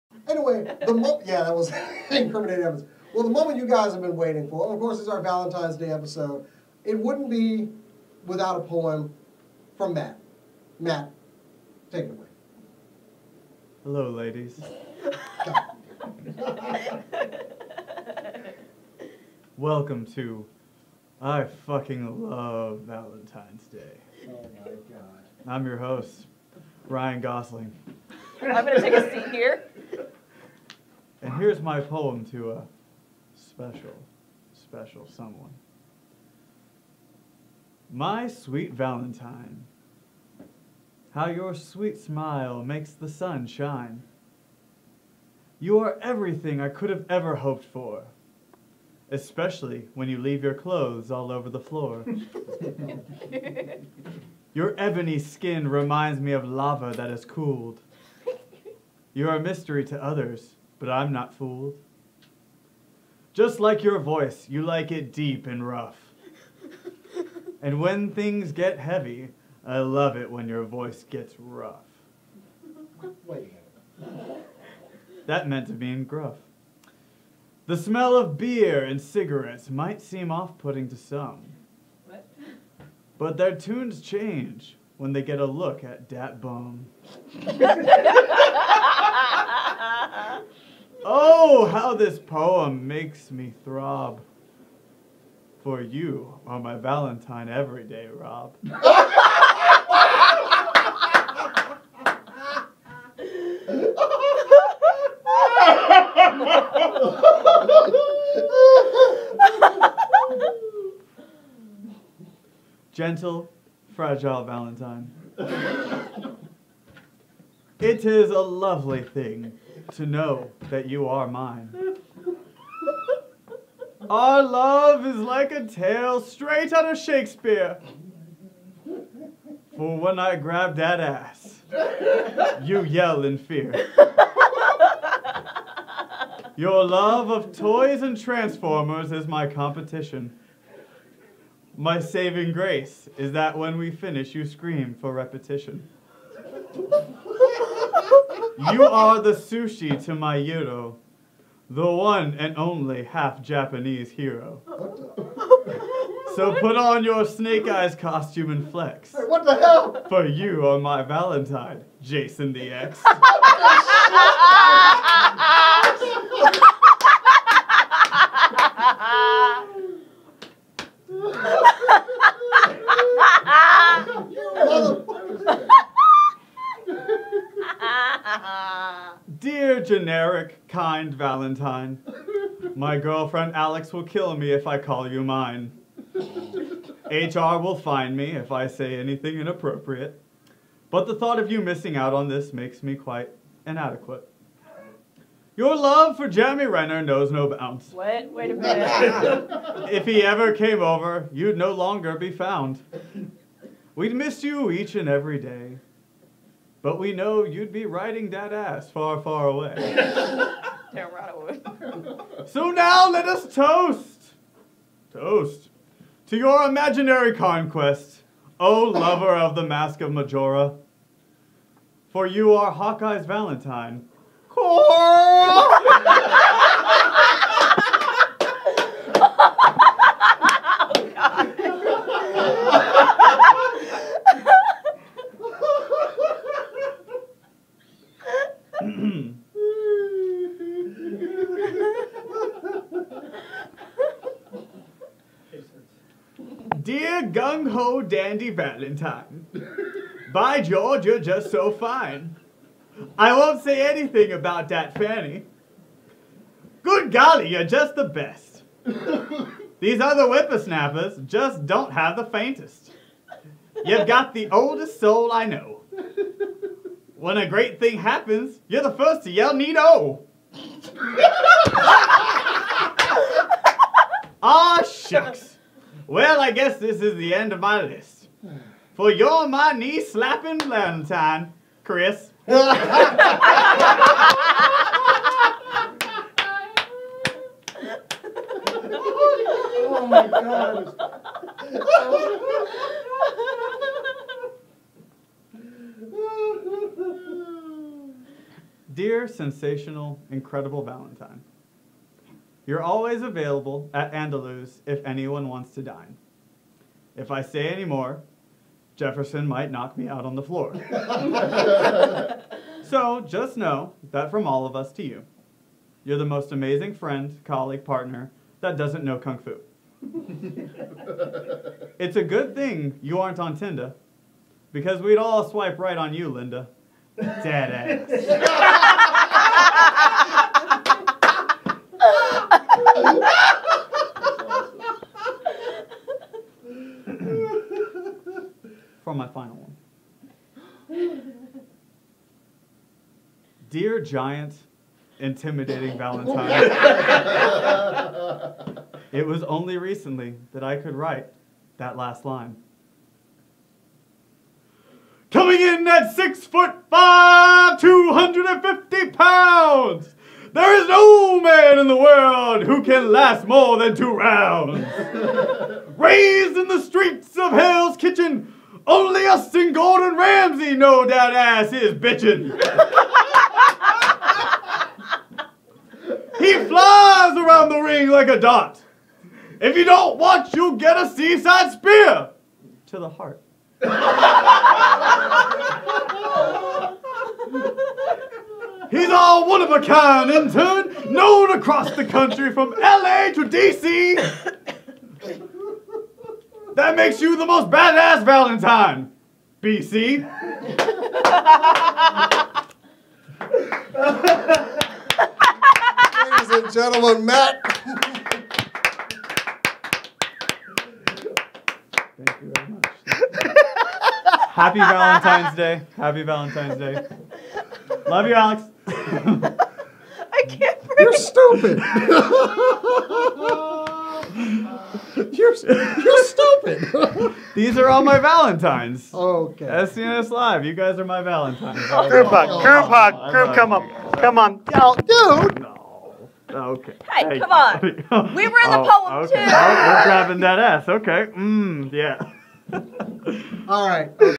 Matt's Valentines Day Poem
In what has become a Valentines tradition Matt gives us another poem.
for the latest on movies, games, and more.